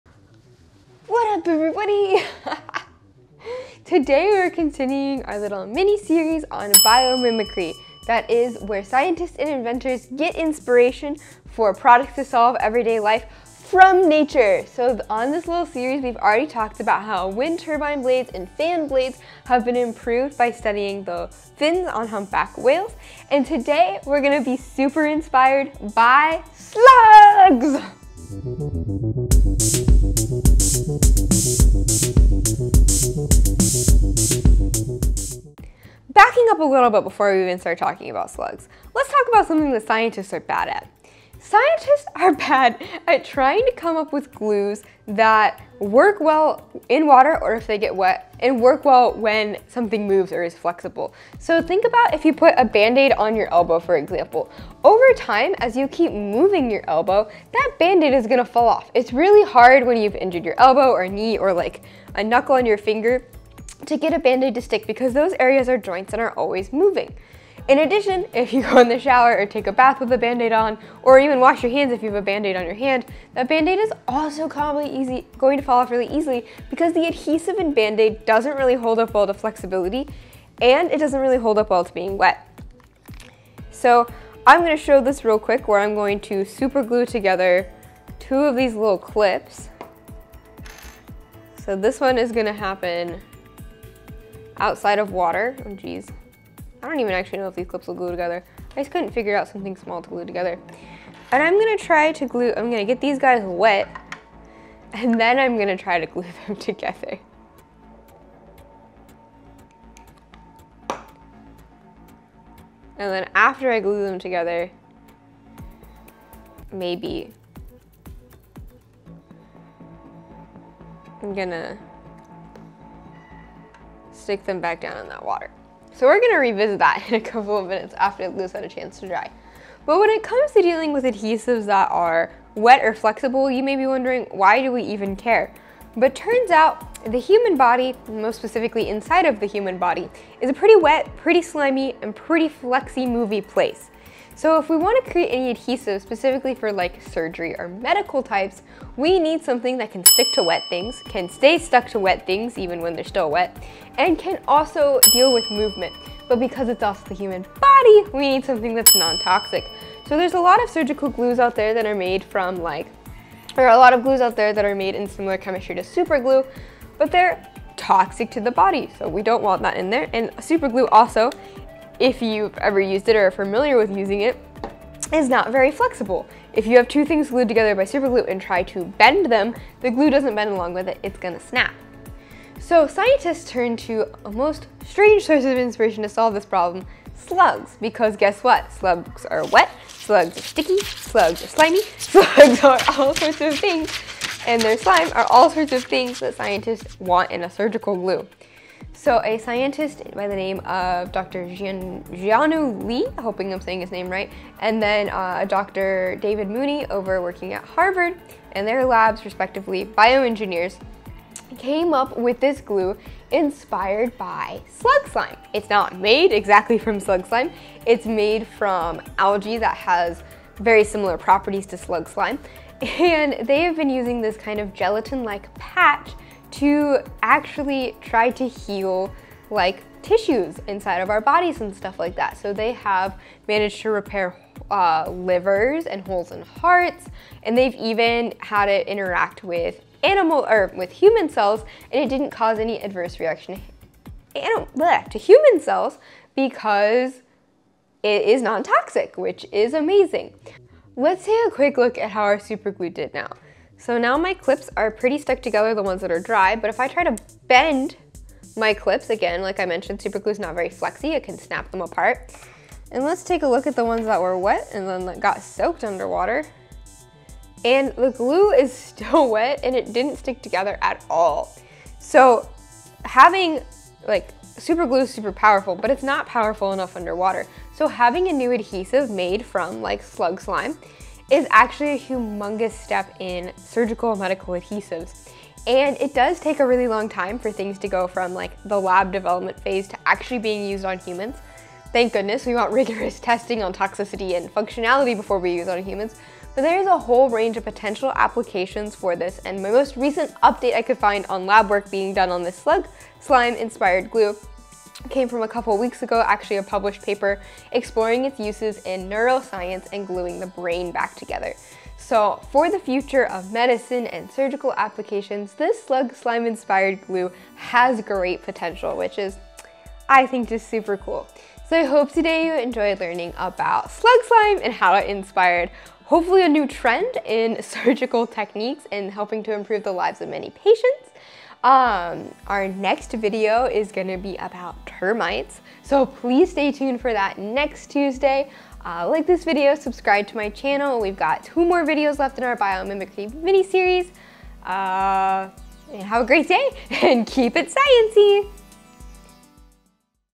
How Slug Slime Inspired Surgical Glue: Biomimicry Pt. 2
We're back with the second installment of our biomimicry mini series! This time it is all about slugs and their ooey gooey slime that can do something that scientists are really bad at...stick to wet things! Inspired by slugs, materials scientists have developed a surgical glue based on slug slime that can stick to wet and slimy organs, be flexible as our bodies move, and are nontoxic to human cells! Amazing! Hope you enjoyed watching and learning!